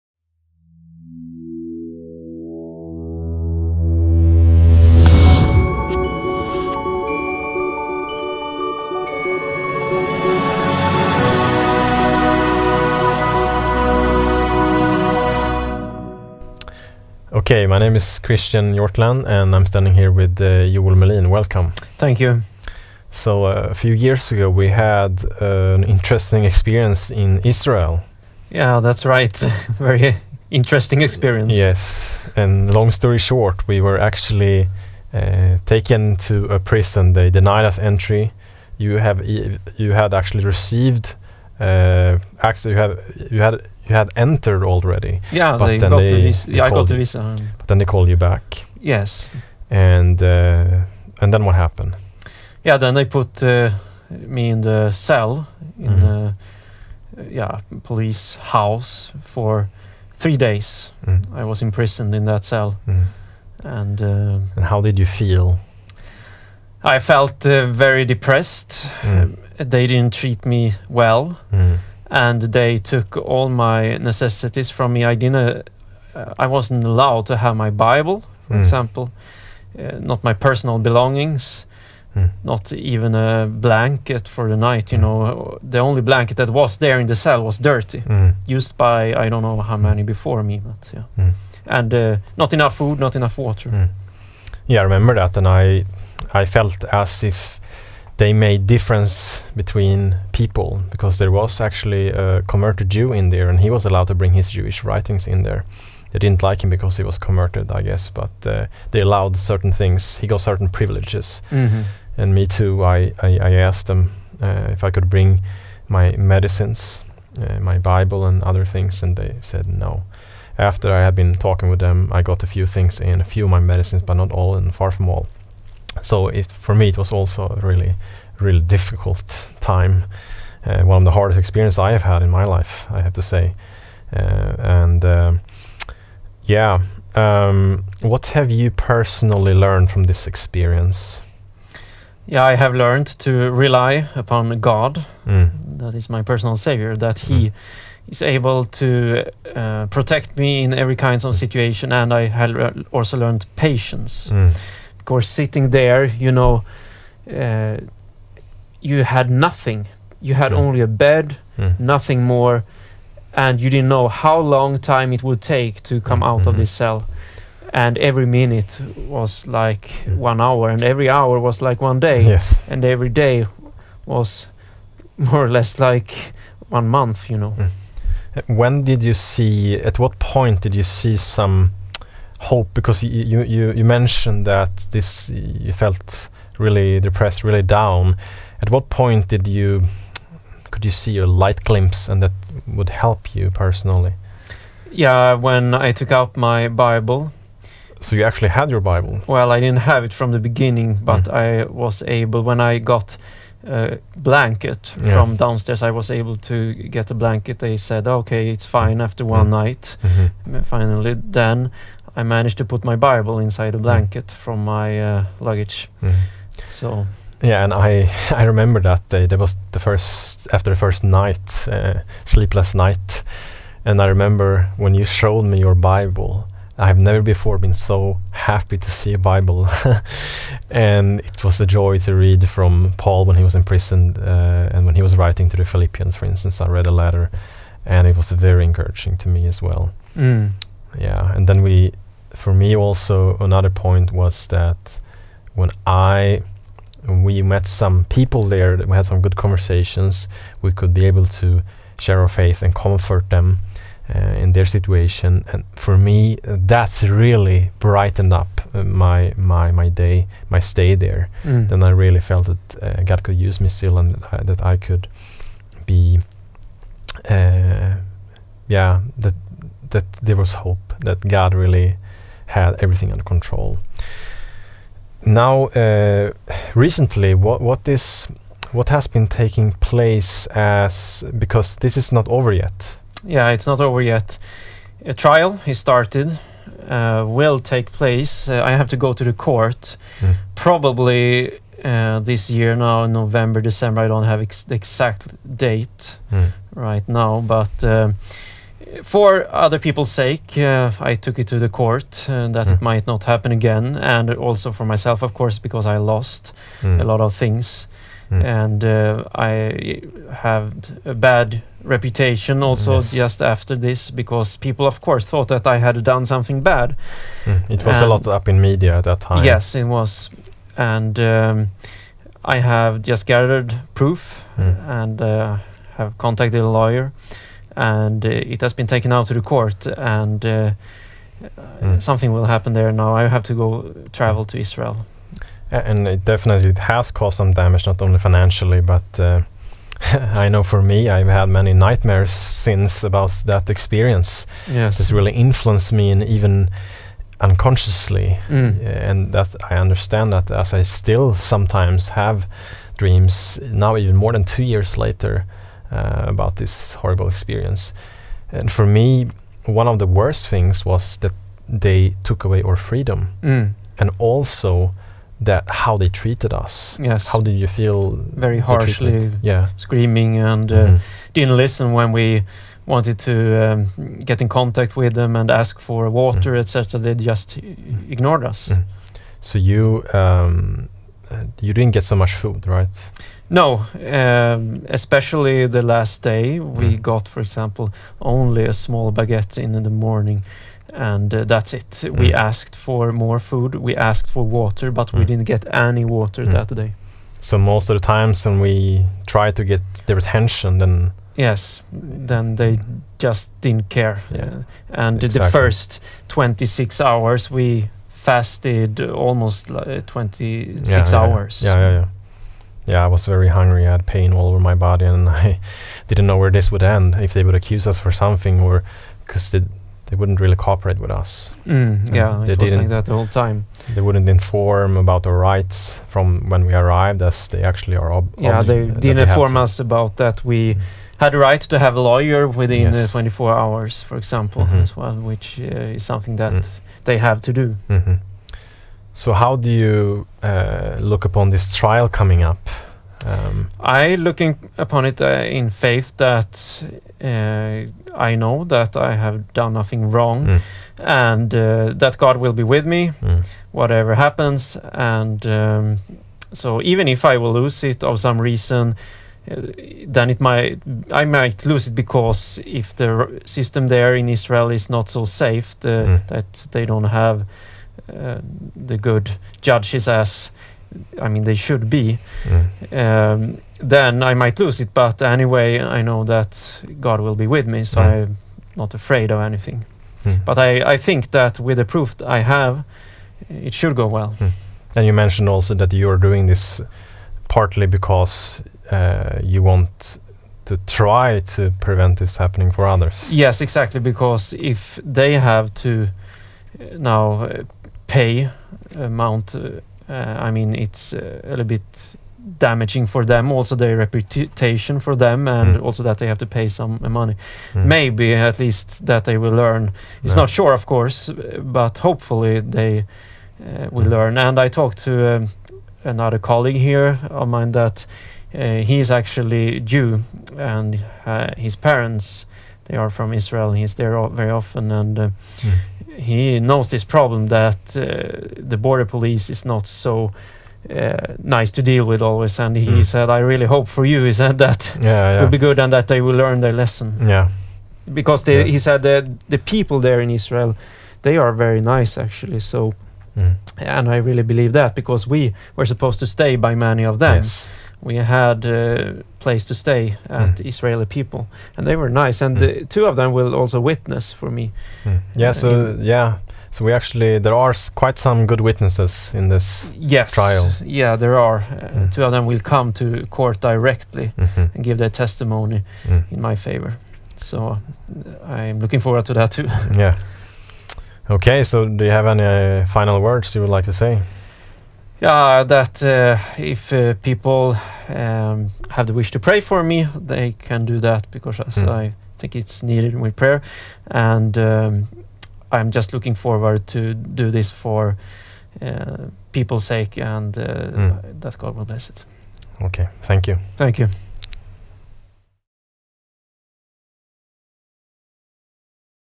Joel Mellin to meet Israeli State in Court after Imprisonment
Joel Mellin and Christian Hjortland were denied entry to Israel in May 2014, and was put in a prison. They got very little food, bad hygiene, and was treated very bad. Hear a short version of the story and why Joel has chosen to take this to court.

Speaker: Christian Hjortland and Joel Mellin